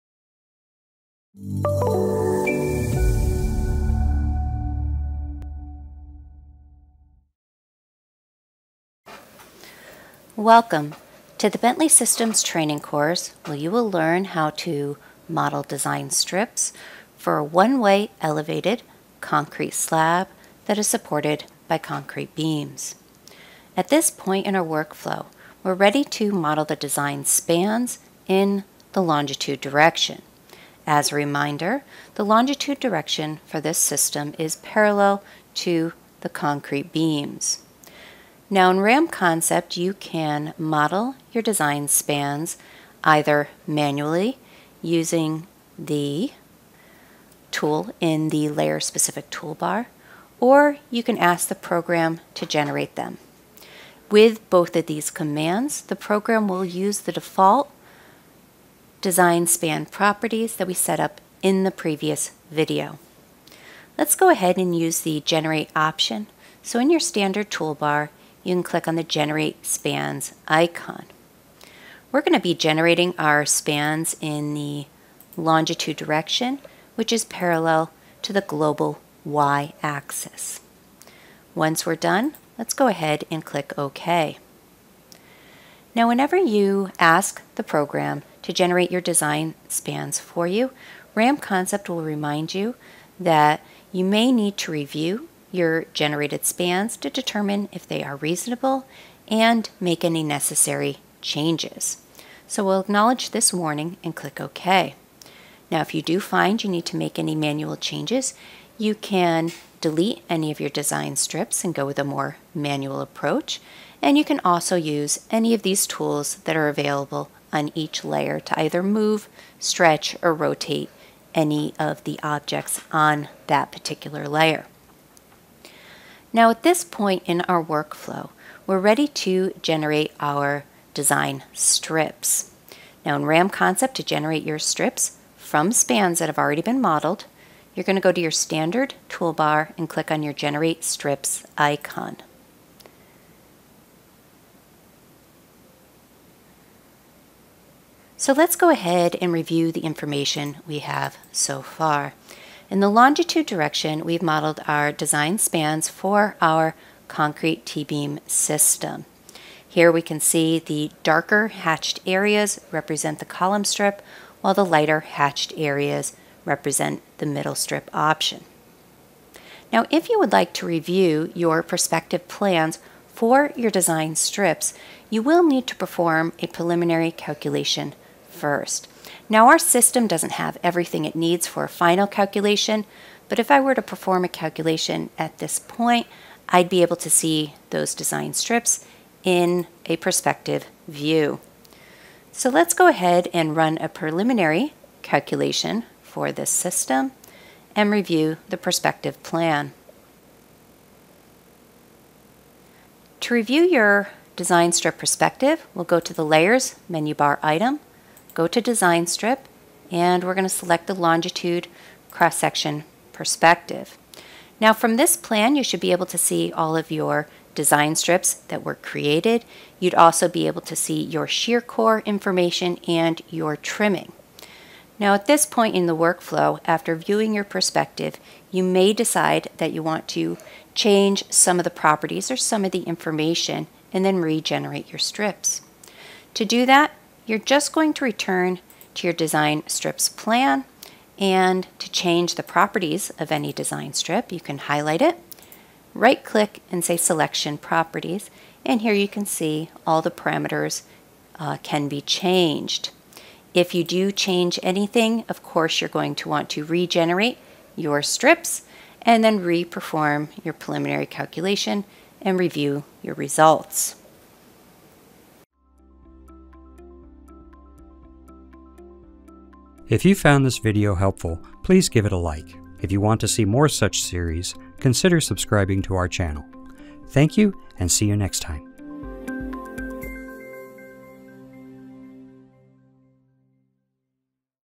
Generating Span Segments and Design Strips for Concrete T-Beams in RAM Concept
In this video, you will learn how to generate span segments and design strips for concrete t-beams in a beam supported, one-way slab in RAM Concept.